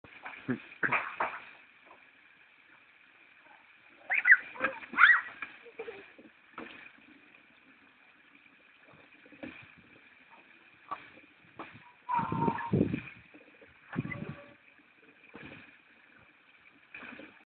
Bike riding
Girls bike riding on the street